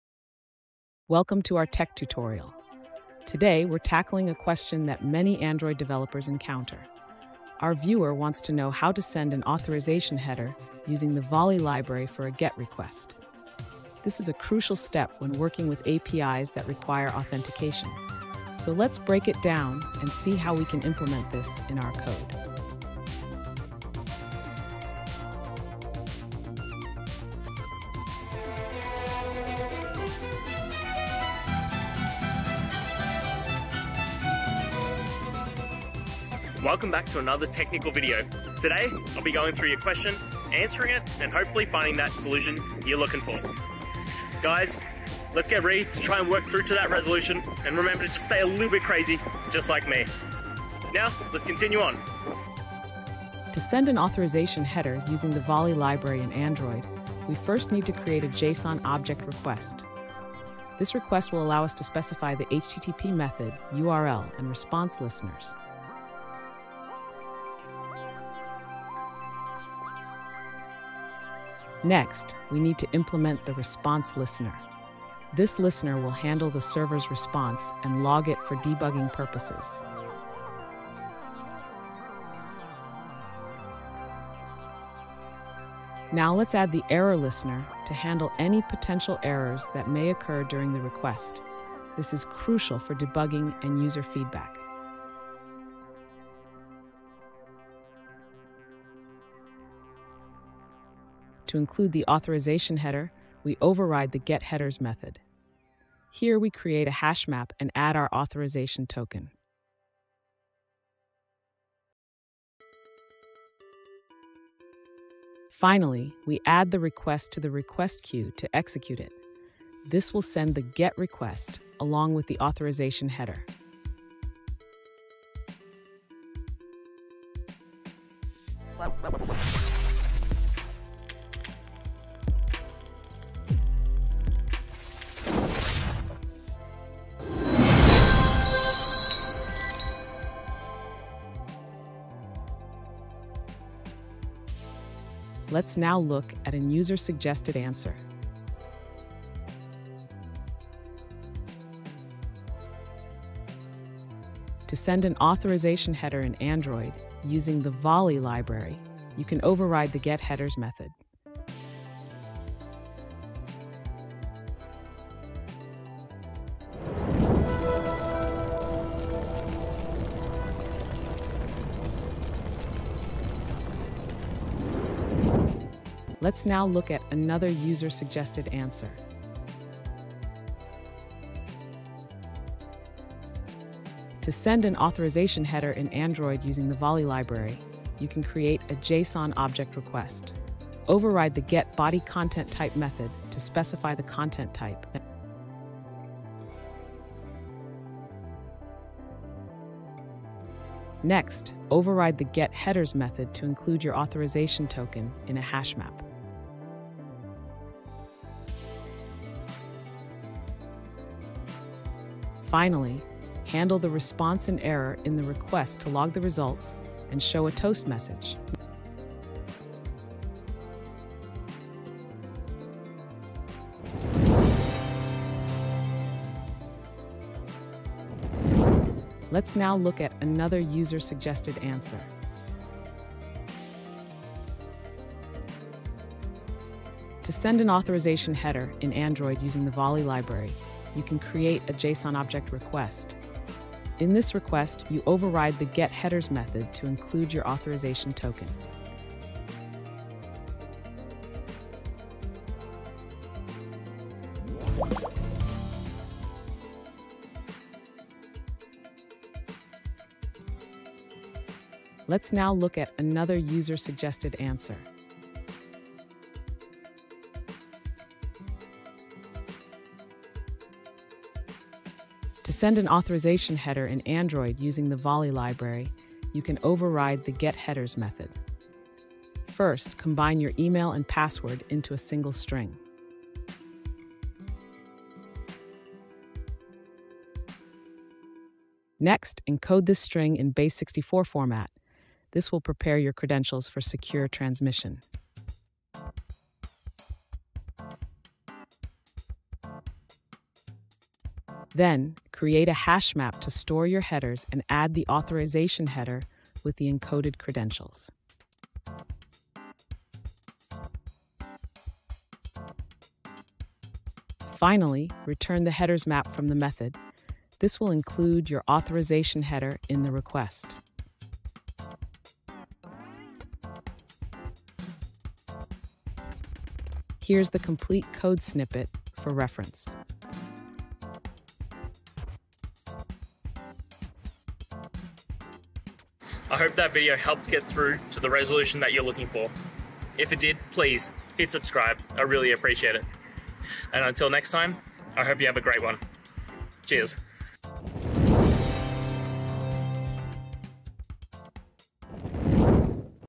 How to Send Authorization Header in Android with Volley Library
In this video, we’ll explore how to effectively send authorization headers in your Android applications using the Volley library. Whether you're working with APIs that require authentication or need to manage secure data transactions, understanding how to implement authorization headers is crucial. Join us as we walk through the step-by-step process, providing you with practical examples and tips to enhance your app's communication with backend services.

Today's Topic: How to Send Authorization Header in Android with Volley Library

Related to